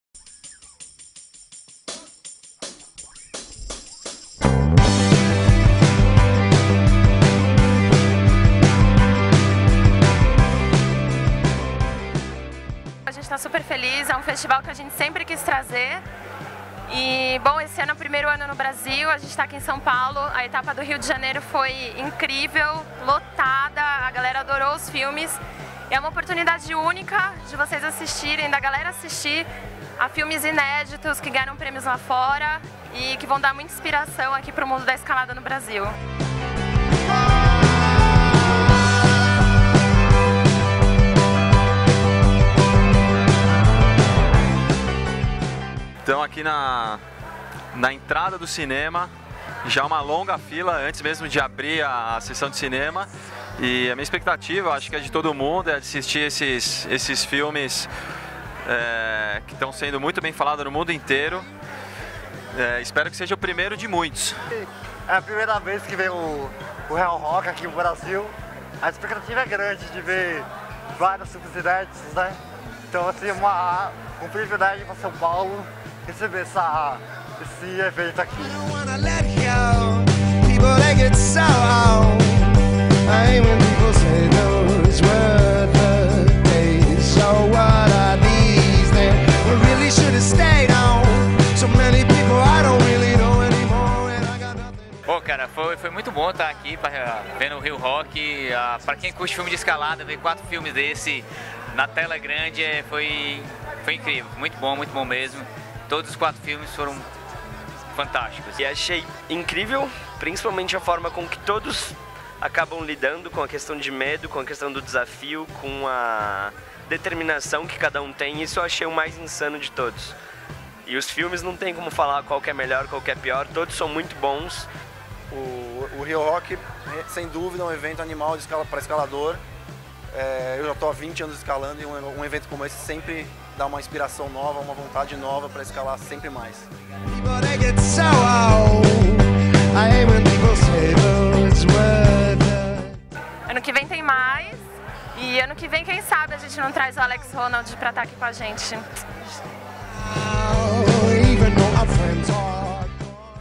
Video reportagem do Reel Rock Brasil
Video reportagem do Reel Rock Brasil - Etapa São Paulo, organizado pelo Blog de Escalada com apoio da The North Face. A primeira vez que o evento foi realizado no Brasil.